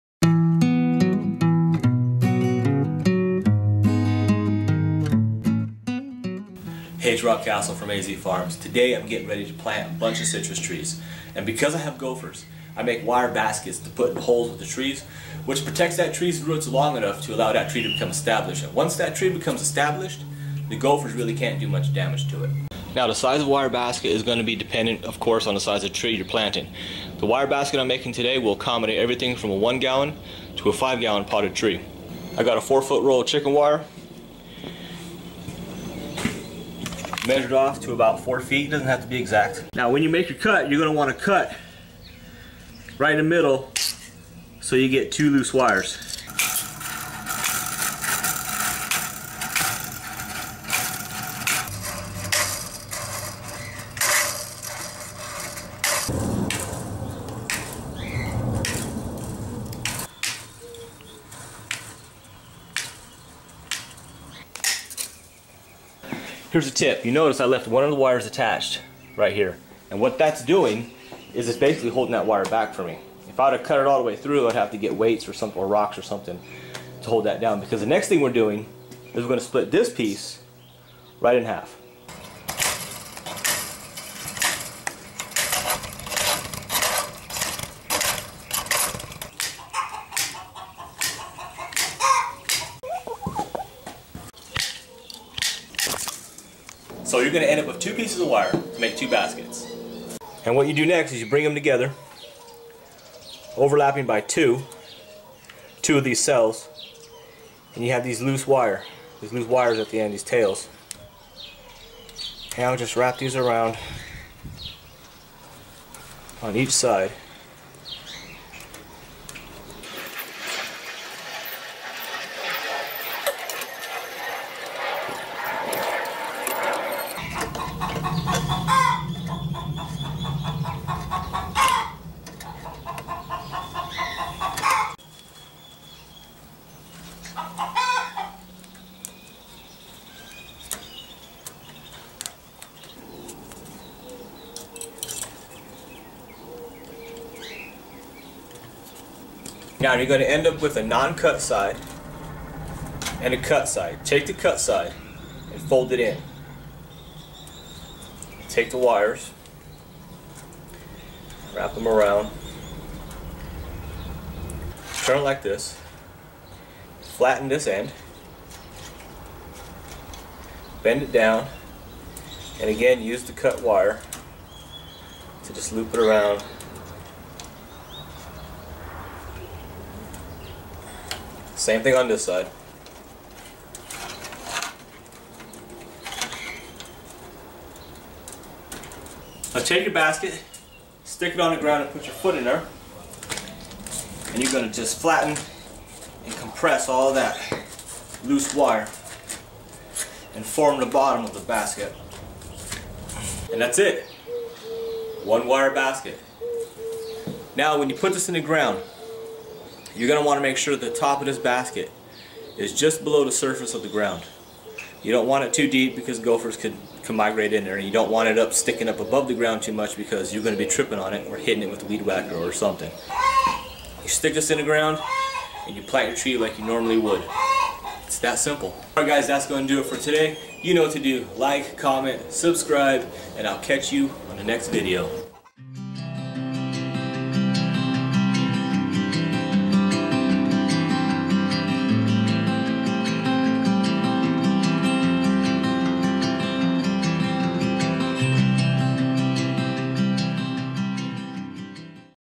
Protect Trees From Gophers
Gophers can cause severe damage and even kill newly planted trees. So I make wire baskets to put in the hole with the tree, which protects the tree's roots long enough to allow that tree to become established and once that tree is established the gophers can't do much harm to the tree. This works not only for gophers but for moles and voles as well.

Top Three Things You Need In Your Soil
--- Azomite Rock Dust
--- Worm Castings